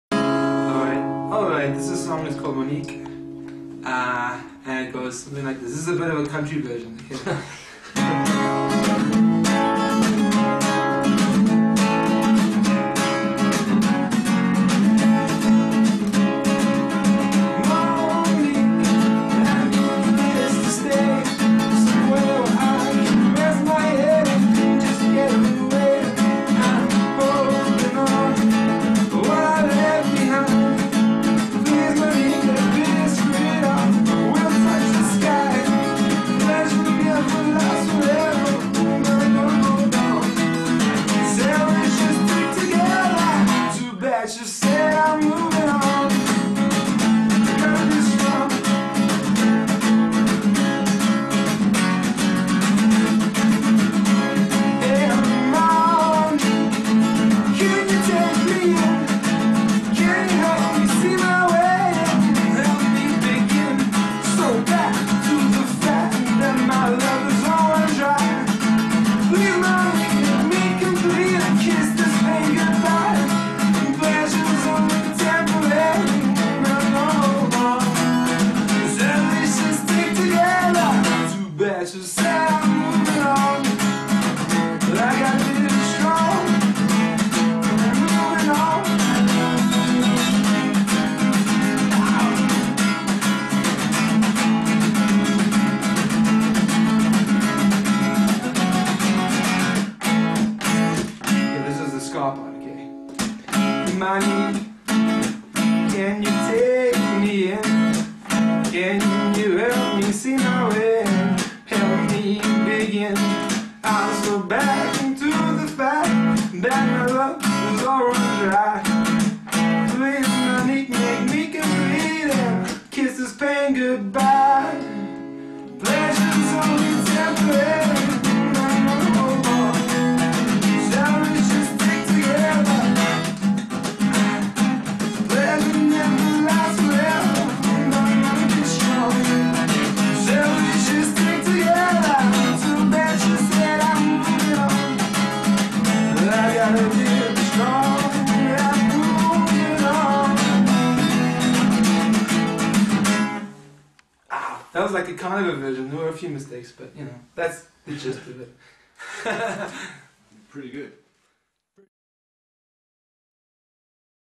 JBones (Fuzigish) Monique - SAPaintball.info Podcast
We got Jbones from the South-African SKA-PUNK band, Fuzigish to spend some time with us. During the interview he played two acoustic songs for us... Enjoy! :)